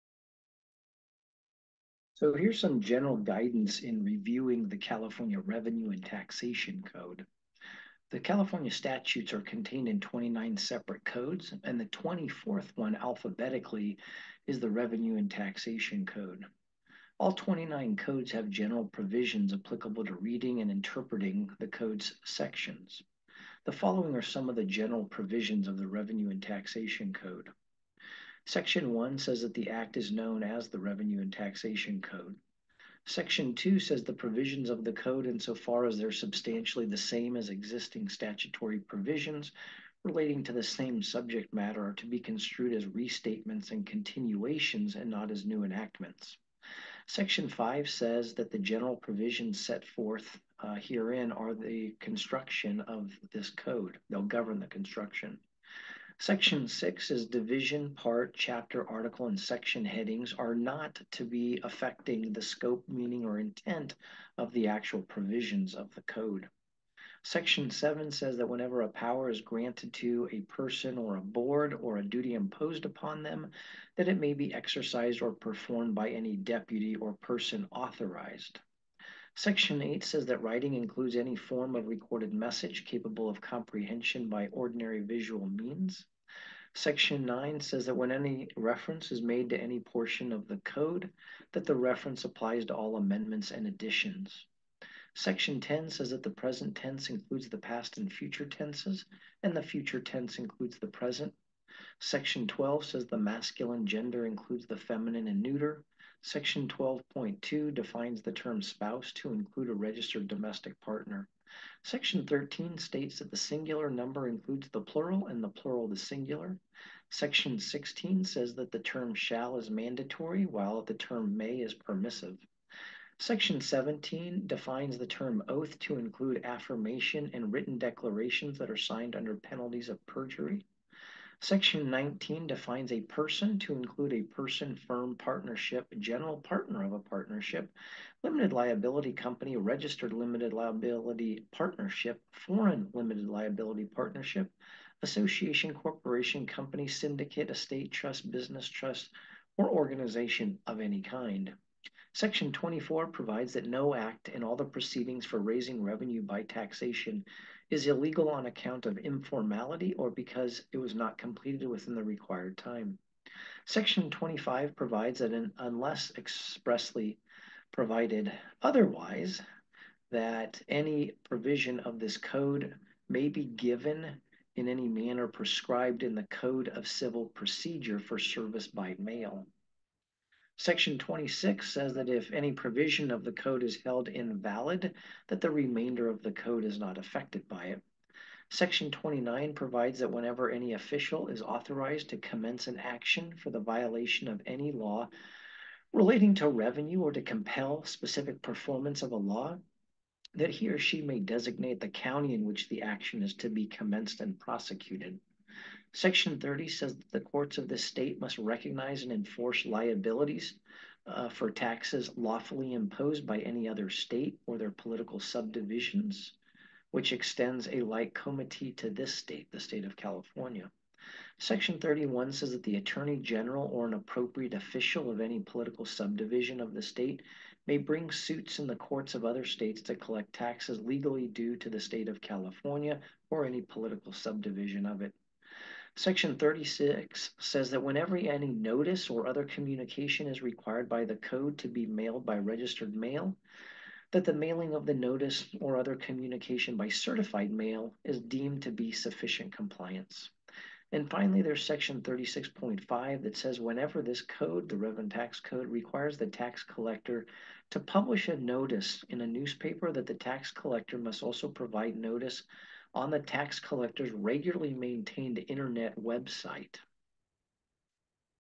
General Guidance on CA Rev and Tax Code
This video discusses some of the general provisions found in the California Revenue and Taxation Code.